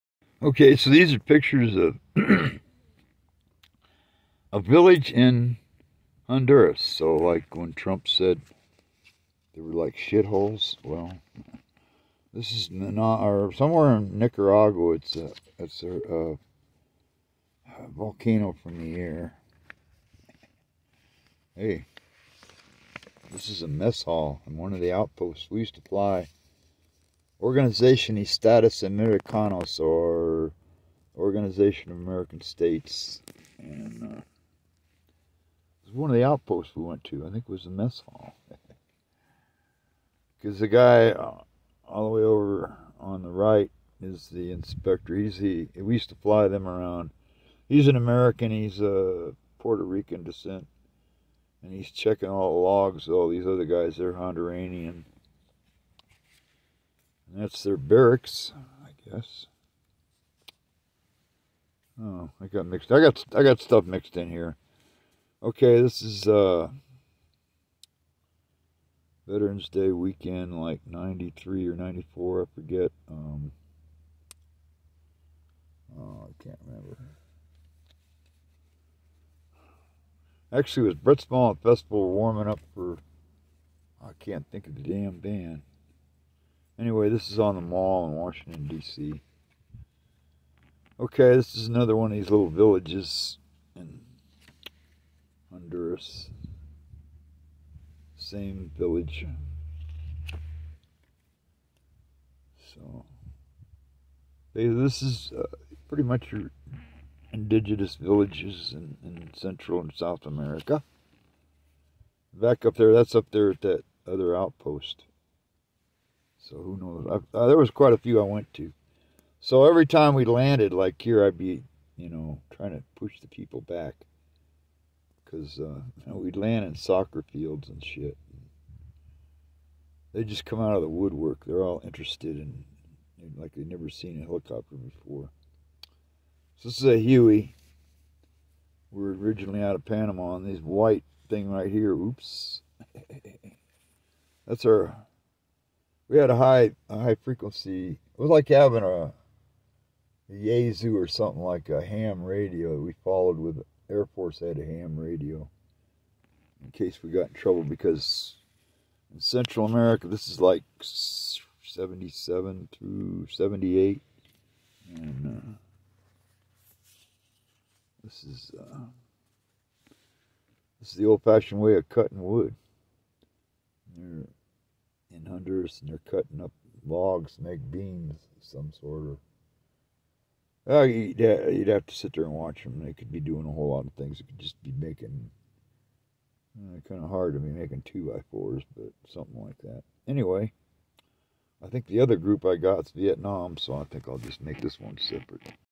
Honduras with Grandpa Rich
from 1977-79 the Panama canal zone to Honduras organization of American States Peace keeping mission